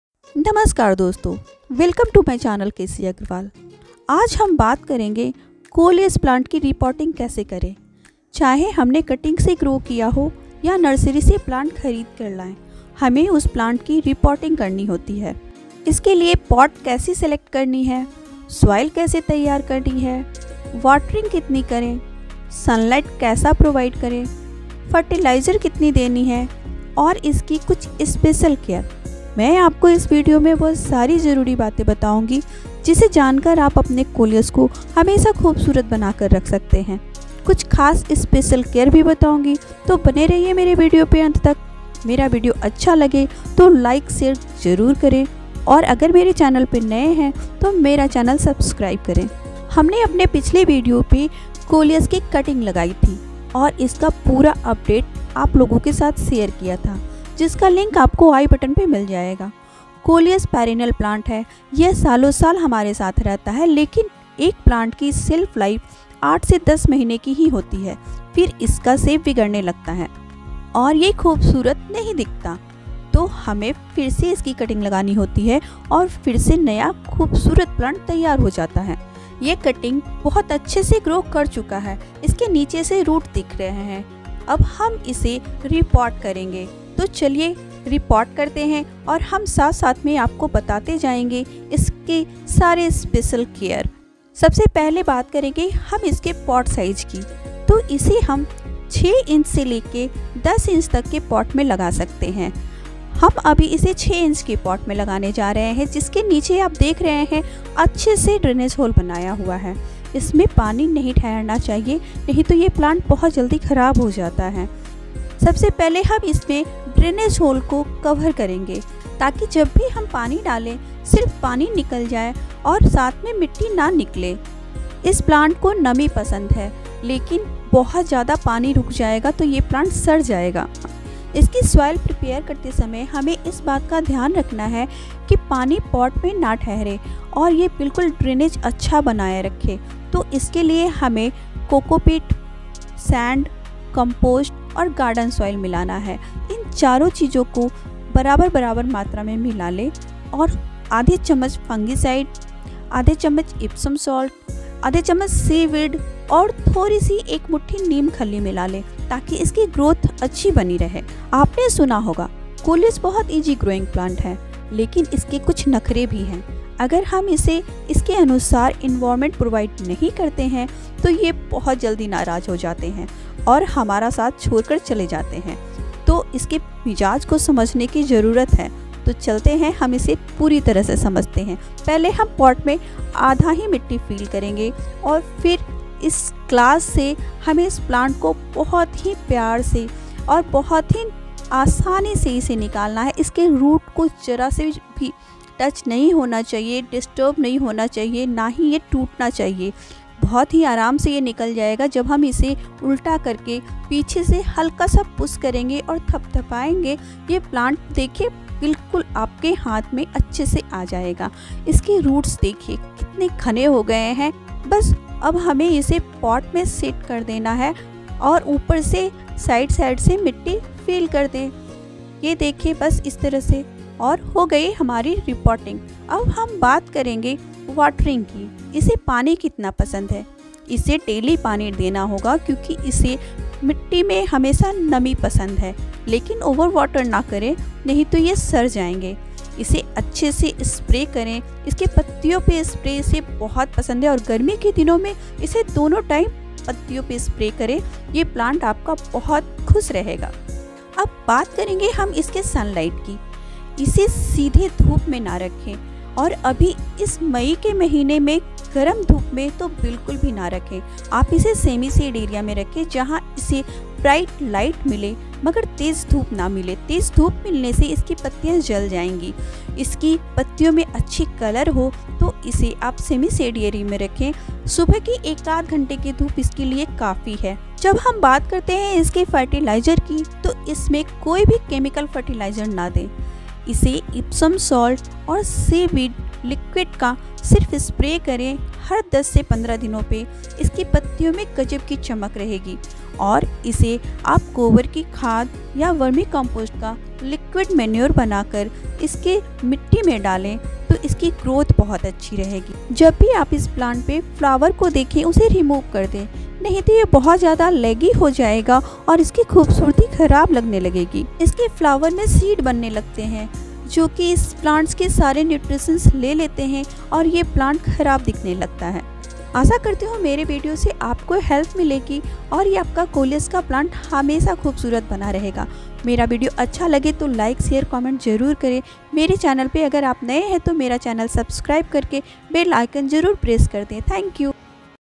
Coleus Care and Reporting |कोलियस प्लांट की देख-रेख |Coleus Plant
Coleus Care and Reporting |कोलियस प्लांट की देख-रेख

नमस्कार दोस्तो 🙏
आज के इस विडिओ में कोलियस की कटिंग लगाना और कटिंग का पुरा अनुभव अपडेट बताया है, आशा करती हुॅ आप सब लोगों को यह पसंद आएगी और कटिंग ग्रो करने मे मदद मिलेगा।
धन्यवाद 🙏